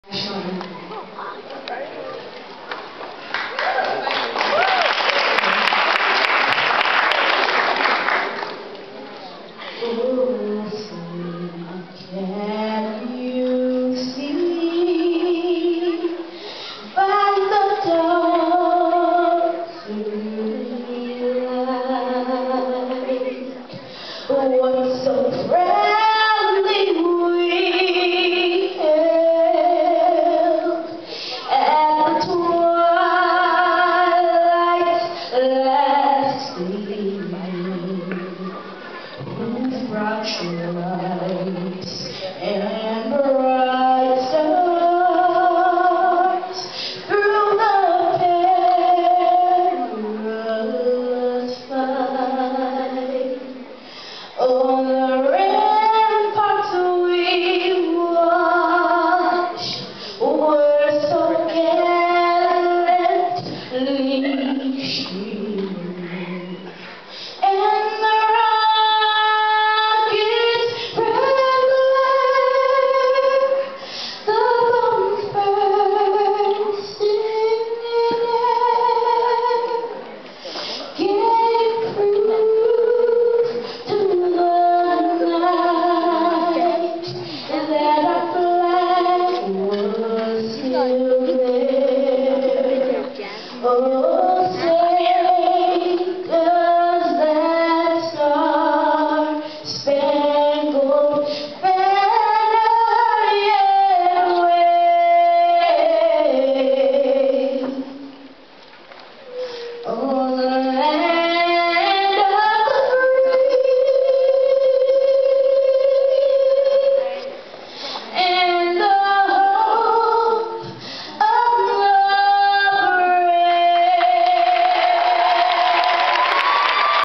At the Beauty Show
Jazmin Brown won the crown...Here is another girl singing...